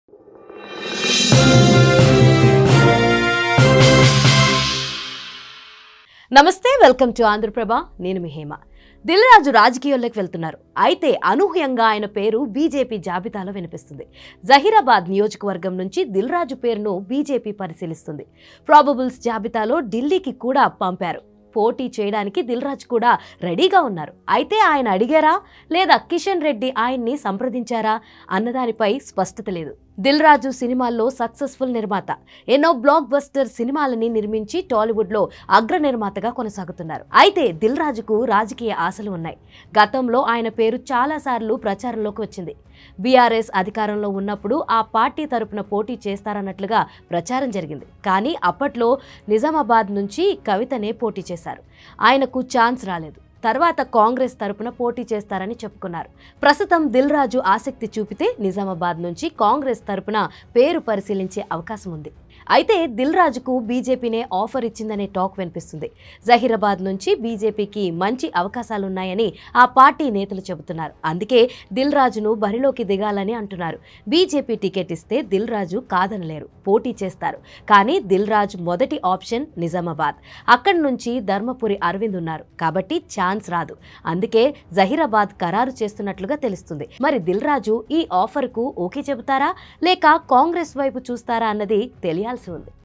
రాజకీయాల్లో రాజు అవుతారా..? | Producer Dil Raju Join Into BJP | Andhraprabha News
Andhraprabha News